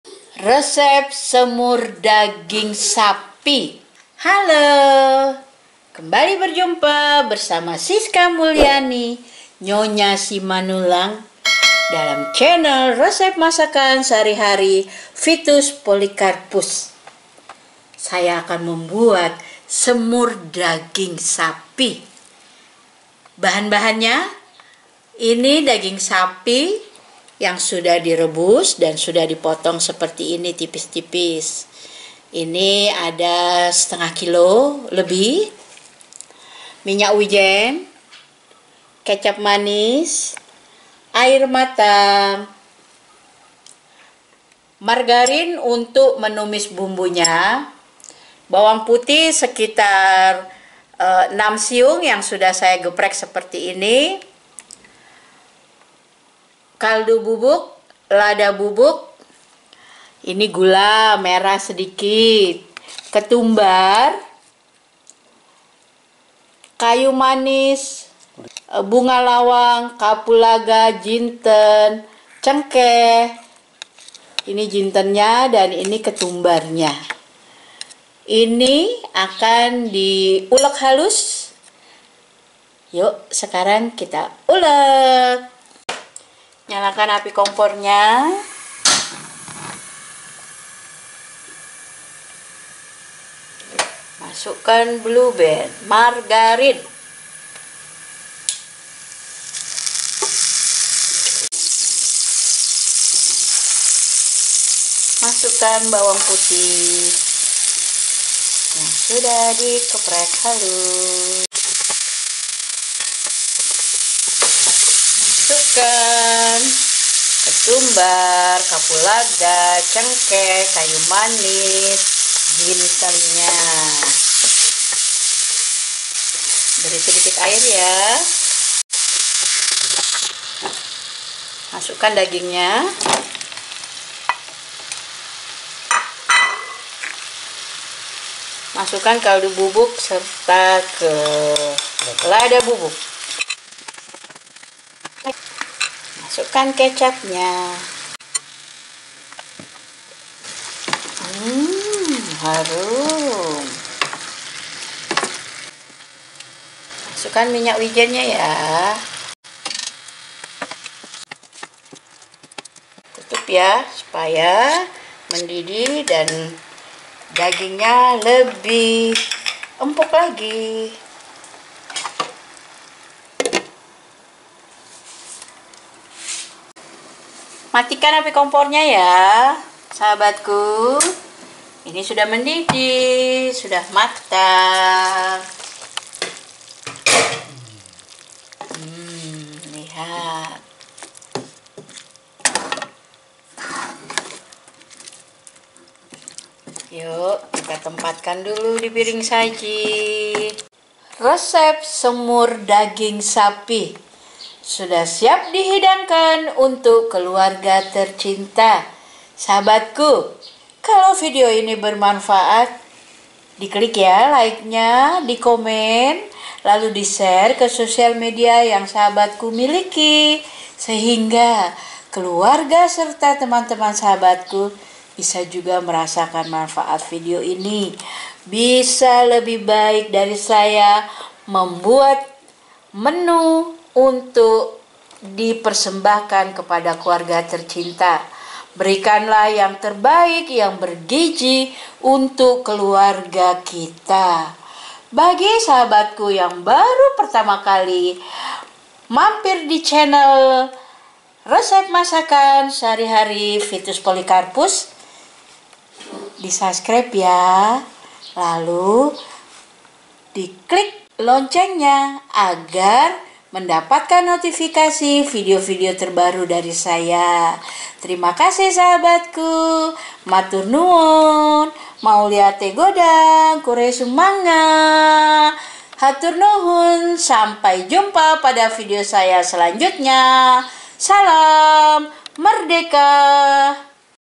Resep Semur Daging Sapi Praktis dan Mudah Cara Memasaknya
Bahan dan bumbu yang dibutuhkan : daging sapi, minyak wijen, kecap manis, air matang, margarin, bawng putih, kaldu bubuk, lada bubuk, gula merah, ketumbar, kayu manis, bunga lawang, jintan, cengkeh
Cara membuat resep semur daging sapi yang enak : rebus daging sapi hingga empuk. Geprek bawang putih, haluskan bumbu. Tumis bawang putih yang sudah digeprek, kemudian masukkan bumbu yang dihaluskan, diaduk rata. Tambahkan air secukupnya. Masukkan daging, kemudian kaldu bubuk dan lada bubuk, diaduk rata. Masukan kecap, diaduk rata. Masukkan minyak wijen, diaduk rata. Ditutup hingga mendidih. Siap disajikan, selamat menikmati.